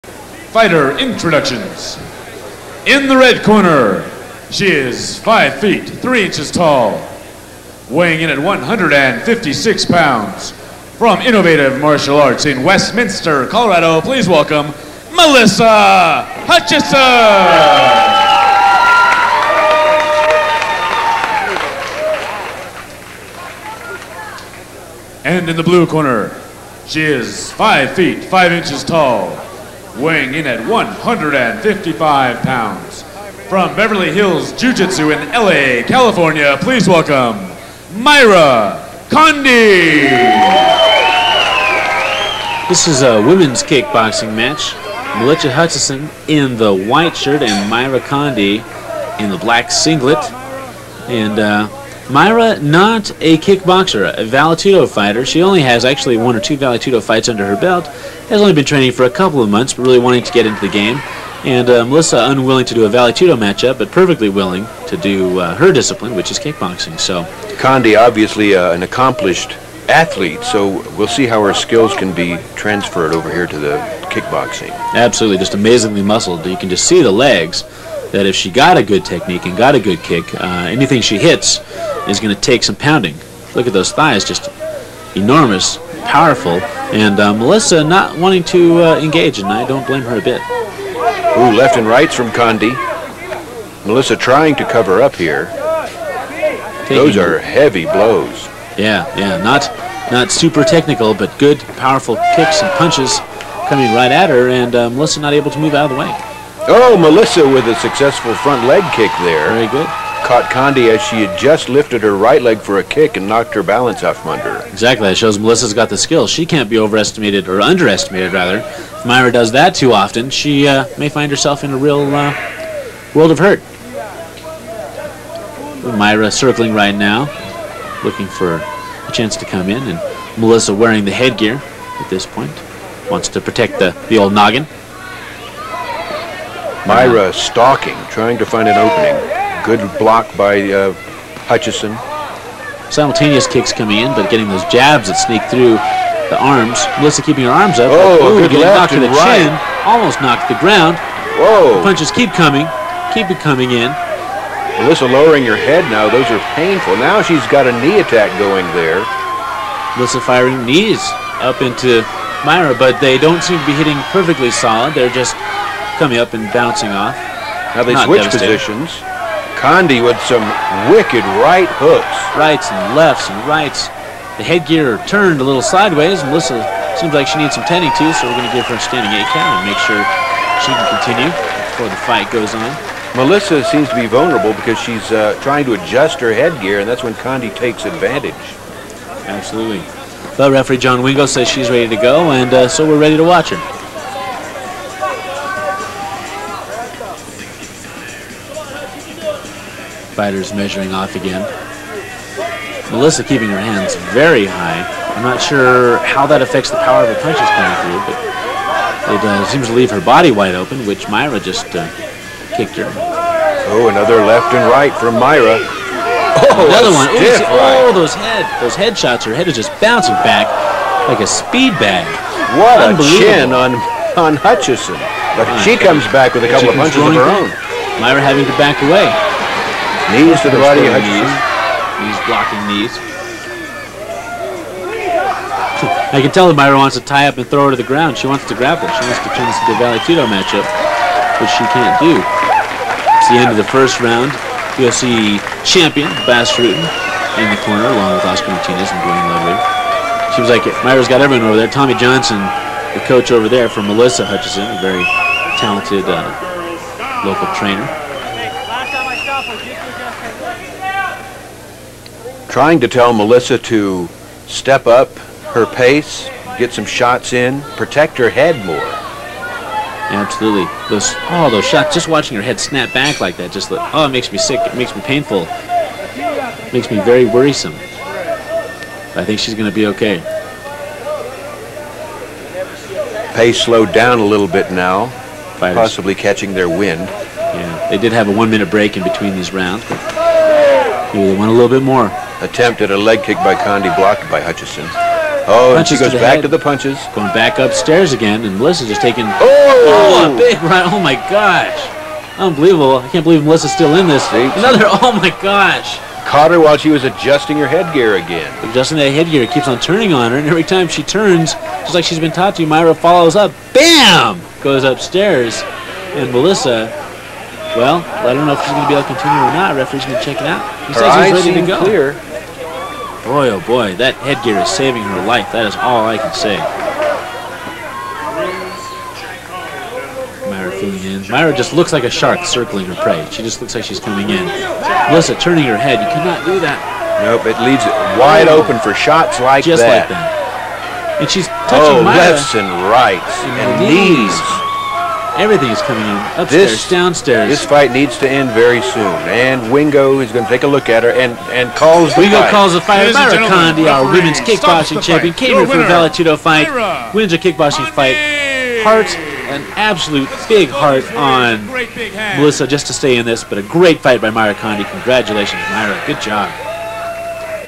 Melissa Hutchinson vs. Mayra Conde (26/06/1999)
Melissa Hutchinson vs. Mayra Conde
Bas Rutten Invitational III
June 26, 1999 | Denver, USA

Light middleweight contest with modified Muay Thai rules.